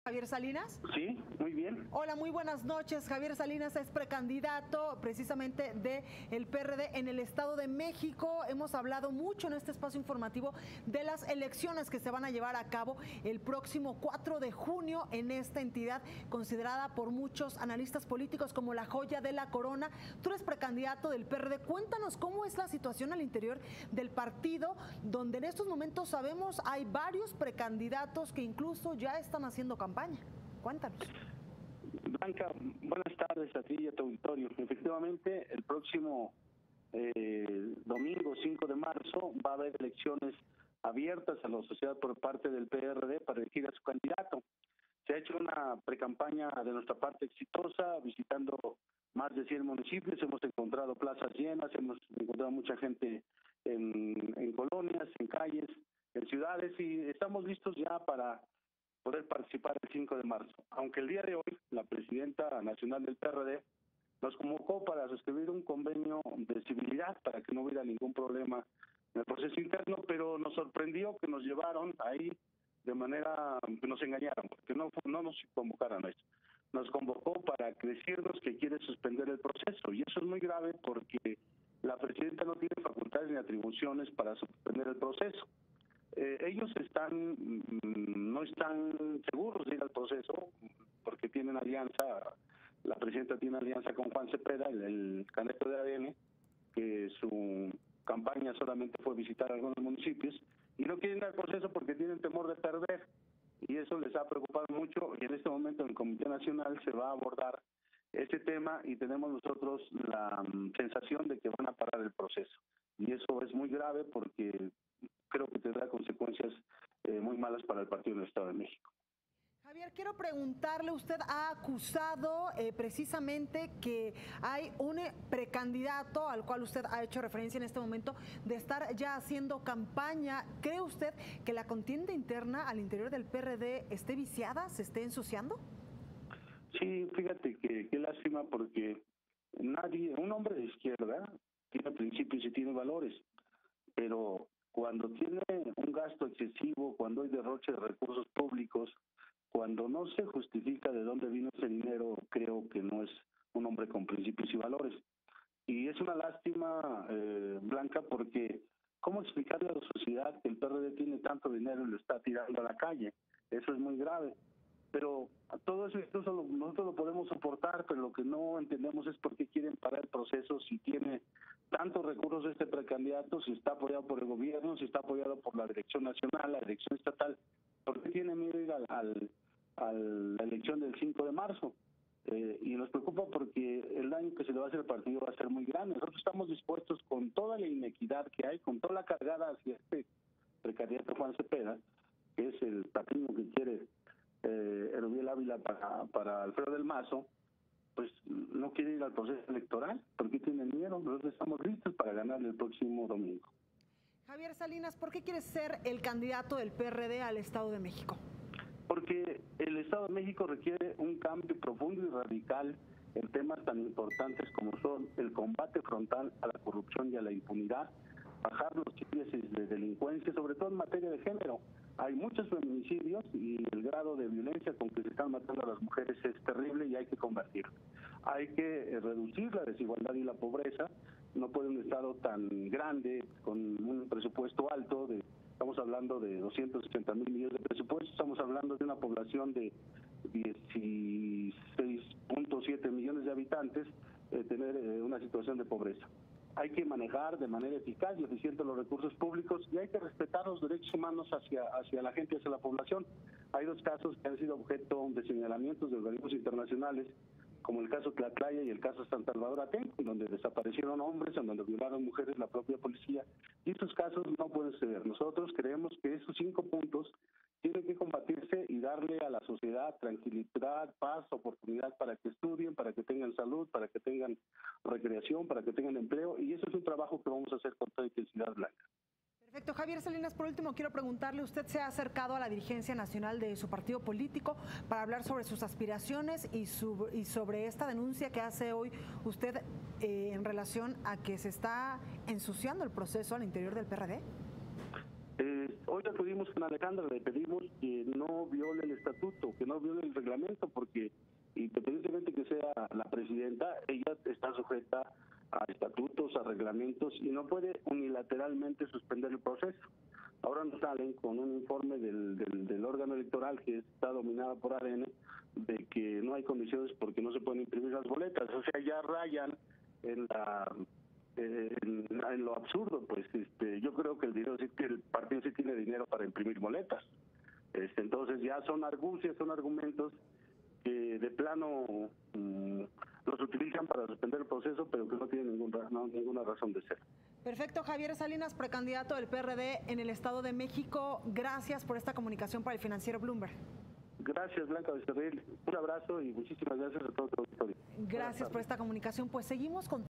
Cómo explicarle a la sociedad que el PRD tiene tanto dinero y lo está tirando a la calle: Salinas
Javier Salinas, precandidato del Partido de la Revolución Democrática (PRD) en el Estado de México. Tema: Candidatura